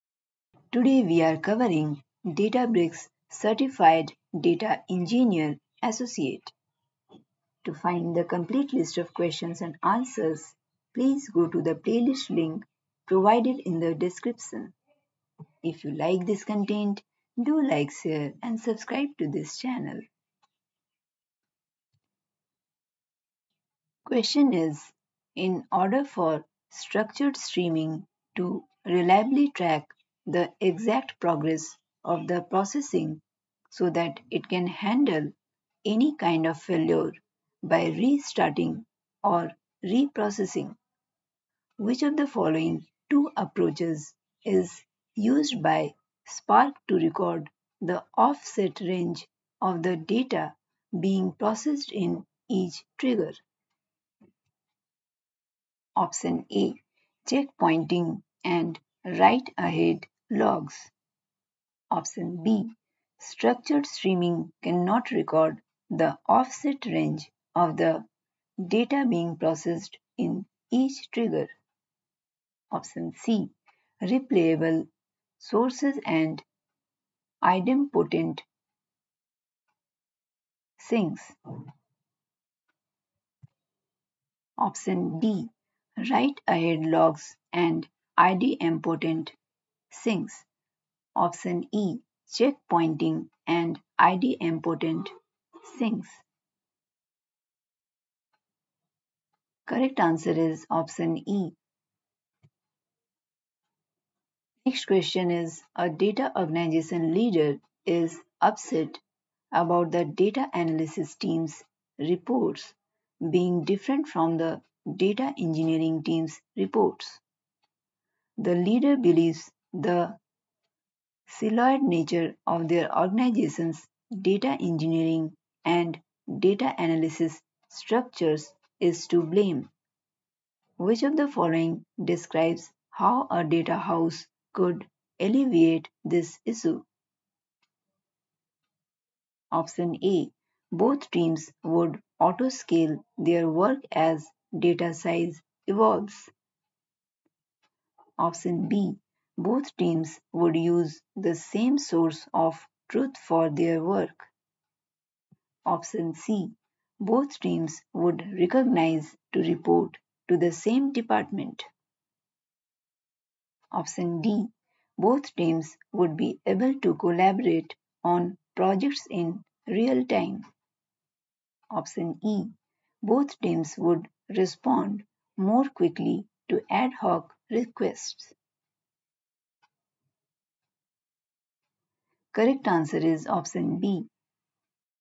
Part#6:Databricks DEA : Databricks Certified Data Engineer Associate - Practice Questions & Answers
**Purpose of these videos is to assist on preparation for these certifications. But always follow the official documentation for accurate information. ***